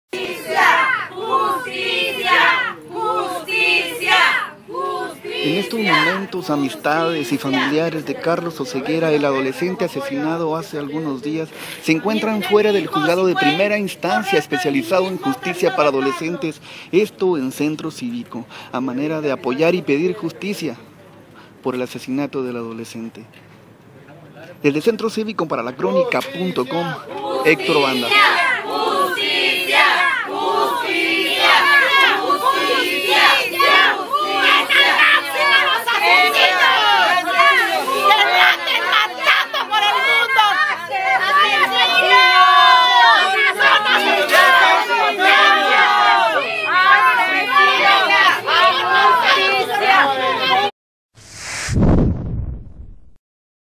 Claman justicia afuera de Tribunal para Adolescentes
Con pancartas y consignas, decenas de familiares y amigos de Carlos "N", claman justicia fuera del Tribunal para Adolescentes en el Centro Civico.  En unos minutos dará inicio el proceso legal en contra de Adrian "N" y Cristina "N", ambos de 16 años de edad. En la acera se encuentran familiares que gritan consignas altisonantes en contra del par de estudiantes implicados en el asesinato del bachiller. Video: Héctor Banda.